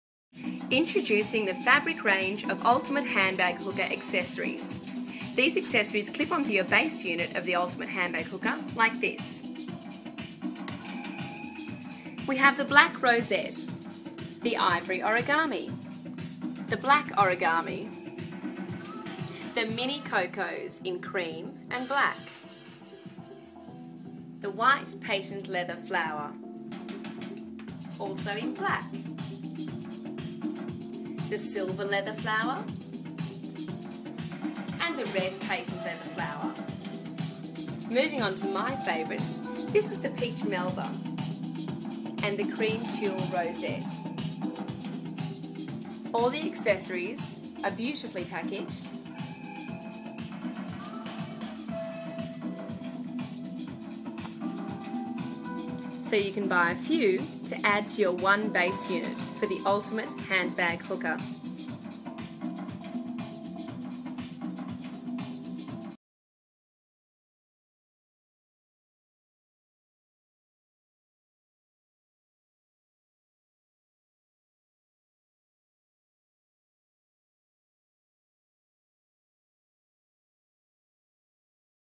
Handbag Hooker - Ultimate. Fabric Flower Range (an IOCO range)
Now the ultimate way to hang your handbag keeping it safe and off dirty floors. Attaches to your bag and has interchangeable accessories. Hold up to 8 kgs and hangs from any flat surface.

Presenting the Fabric Flower Range